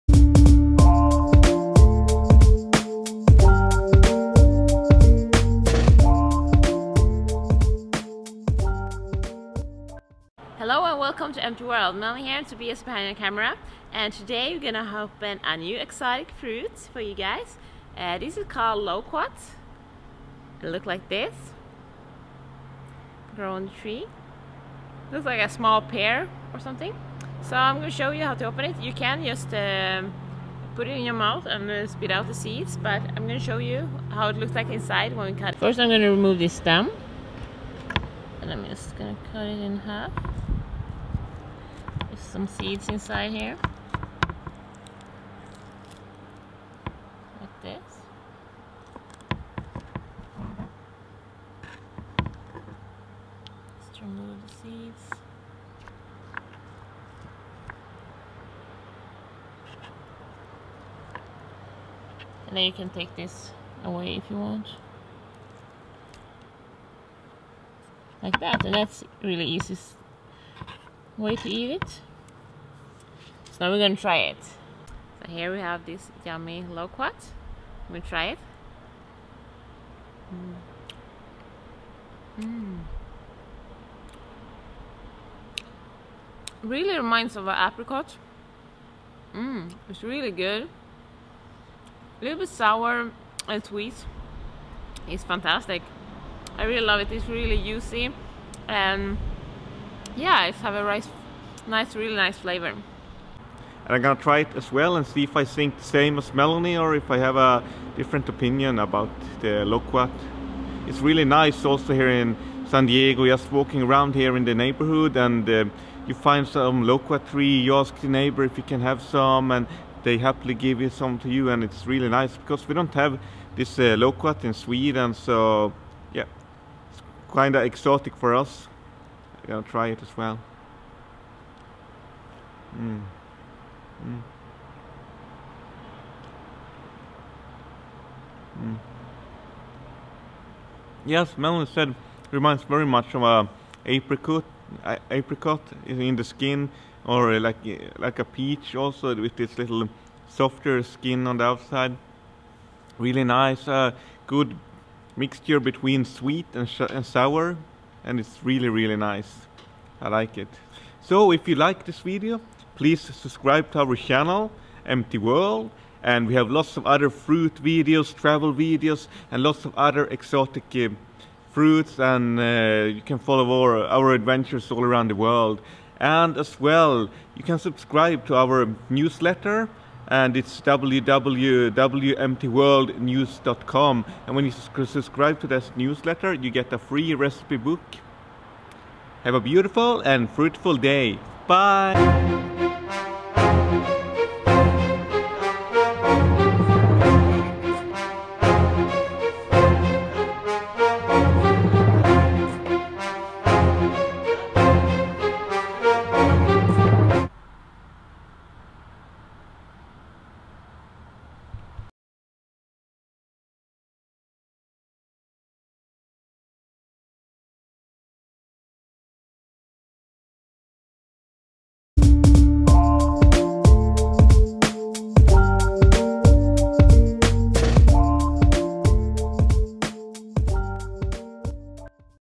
How to open and eat Loquat fruit
MT World are in San Diego and found a big Loquat tree. Loquats are not so common fruit, but very delicious, many people do not know what it is and how to eat it, so in this video we going to show you how. Loquats have many health benefits, check the video for more information.

For blogs and more information about different fruits check our homepage
and join our newsletters and get our free raw vegan recipe book